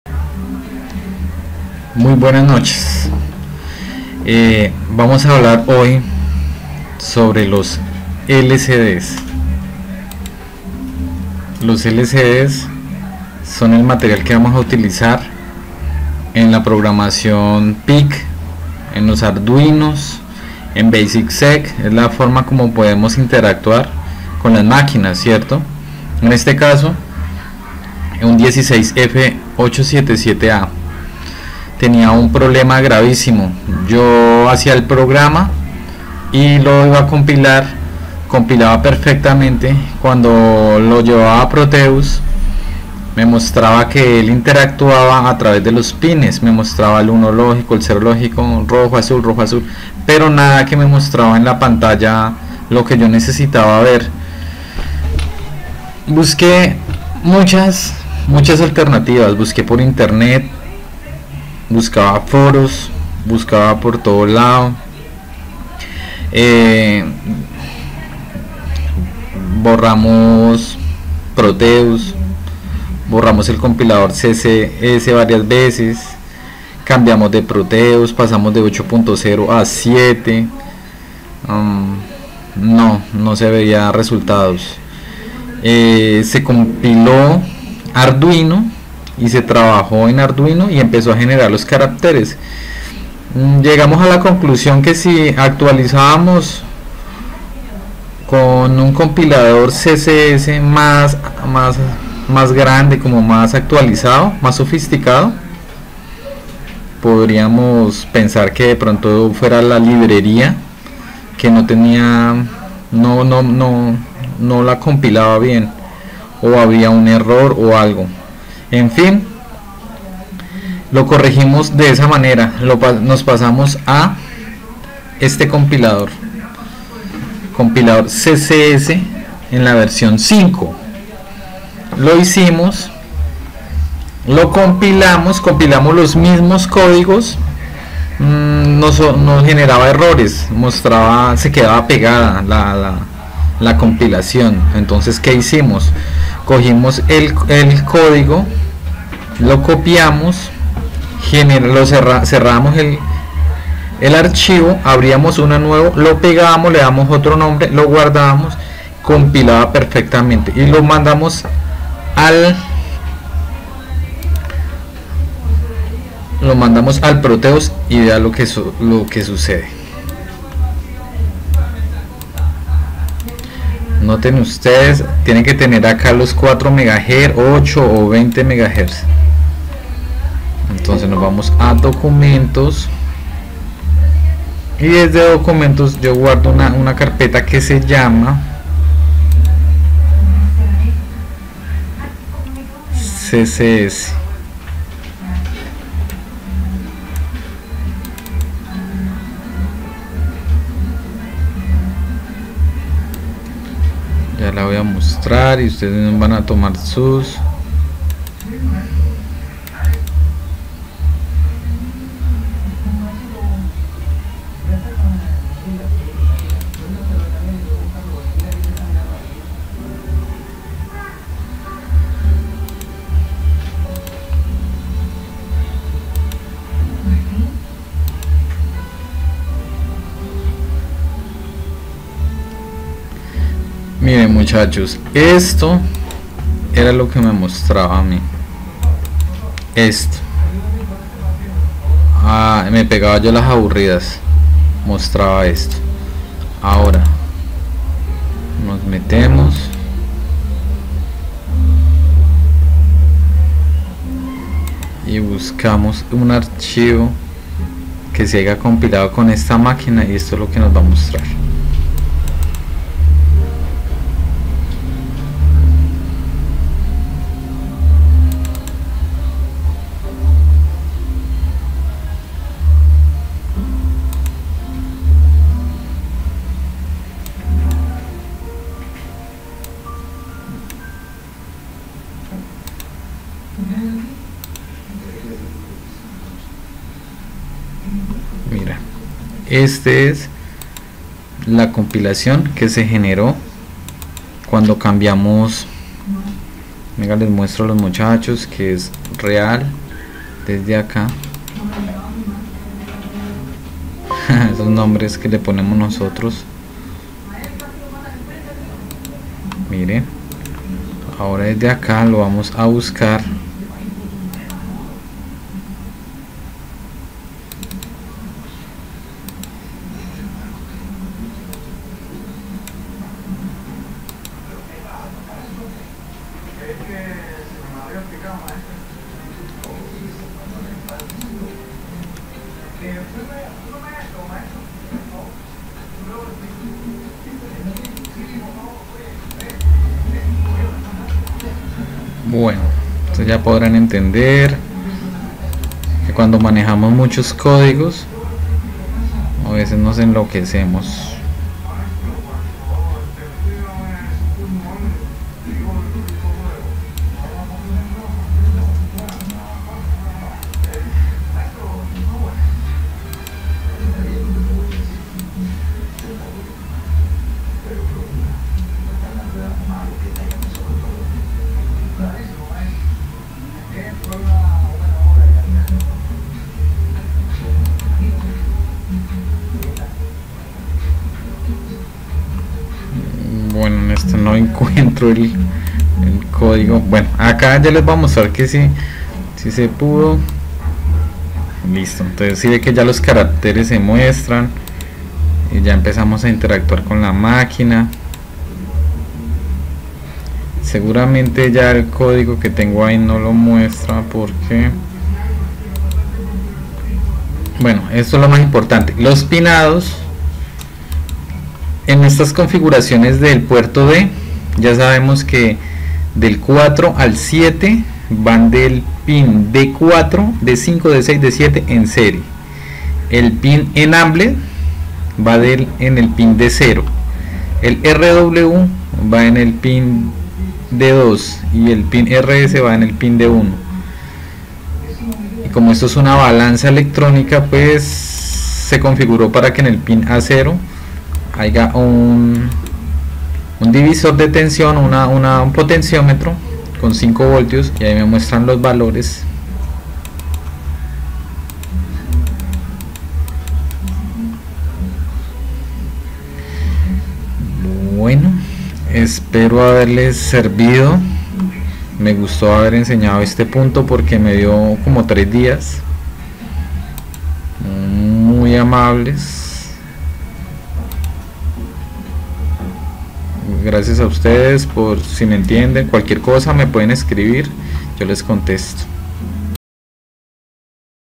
problemas con lcd en proteus,mira la solucion.
el siguiente video fue creado ya que el lcd 16x2 no mastraba nada en proteus, se da soluccion
cambiando de compilador por uno mas actualizado